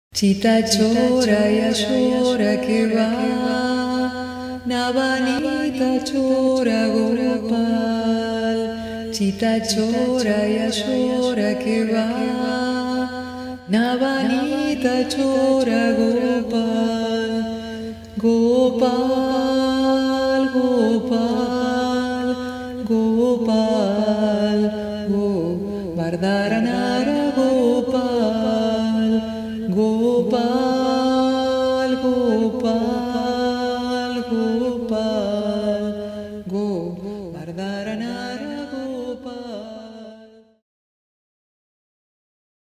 Mantras & Kosice IV
Un viaje:  relajar la mente y dejarse sorprender por la obra del genial creador, Gyula Kosice.  El sonido del agua se entremezcla con mi voz al cantar Mantras de la India. Invita a la meditación.